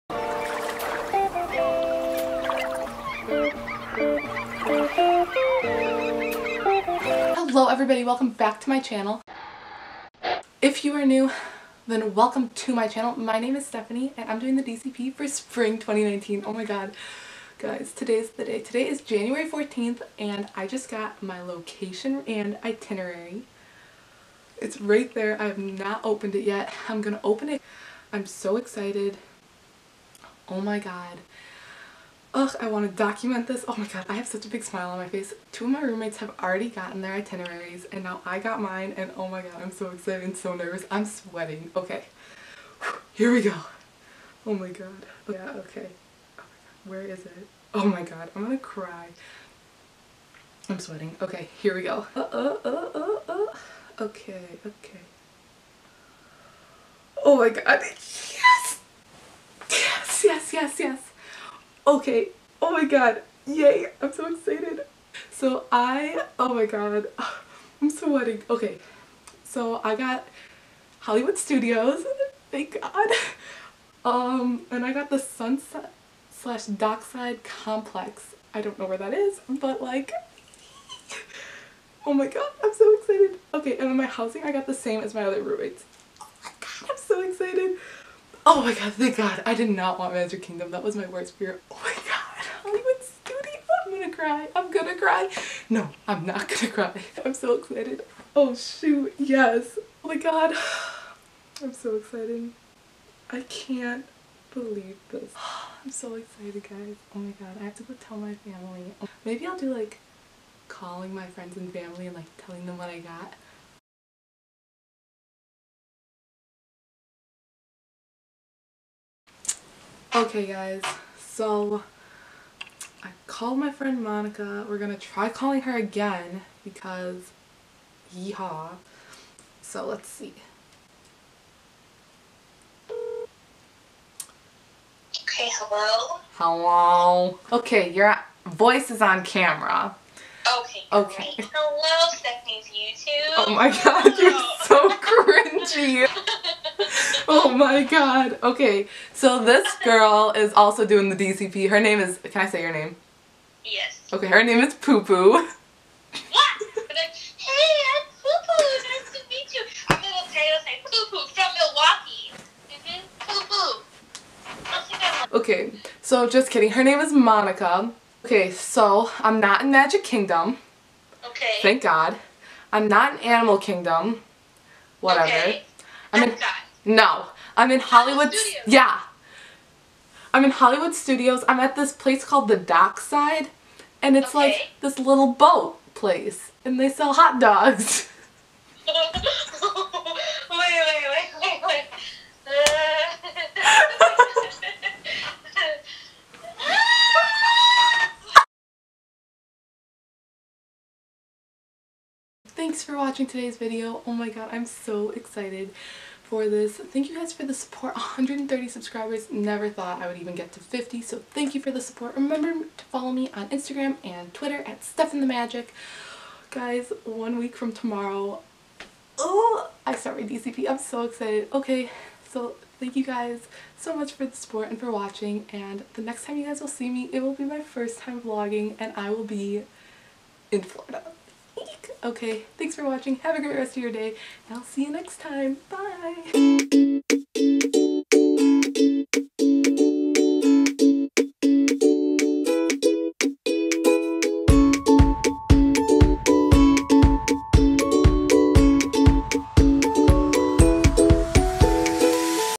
location reveal ♡ | dcp spring 2019
I finally found out my location for my program! Thank you all for the support and for watching today's video. I'm so excited for this new adventure to start NEXT WEEK. Get ready because I start vlogging THIS WEEKEND!